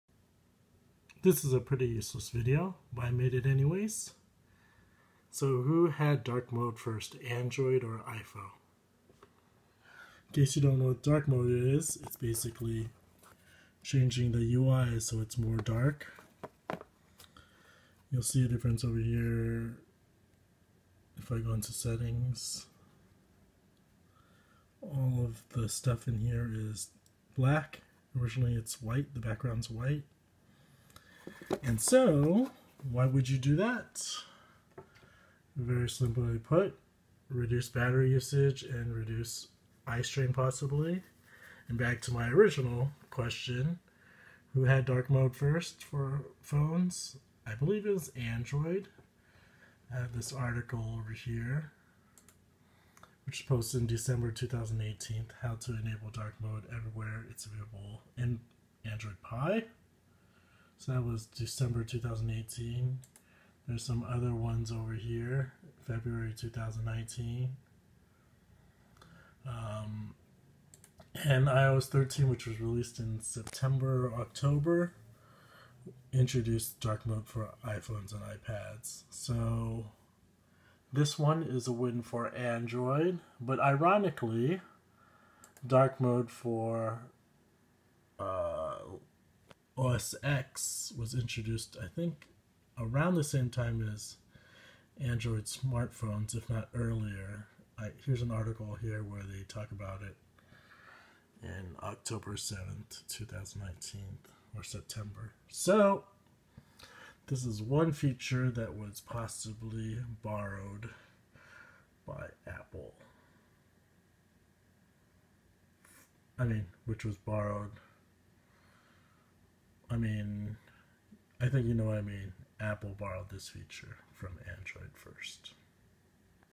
Which smartphone had dark mode first? Android or Iphone? I think Android 9 PIE in 2018 !
In my series where I examine who had it first.  It can be argued that android has been on the leading edge of innovation.  One of these innovations is dark mode.

Why have dark mode?
Save battery.
Easy on the eyes (and eye strain).

So I believe dark mode for android was instituted somewhere around android pi 9.  (on older android's you can enable it partially using accessibility "High Contrast" Mode.)  This occured around end of 2018.

Well, iphone users finally got it as an official update in ios 13.  This was released around august/sept 2019.

Yep, so in this battle of innovation, I'm calling it Android that first came out with darkmode for smartphones!.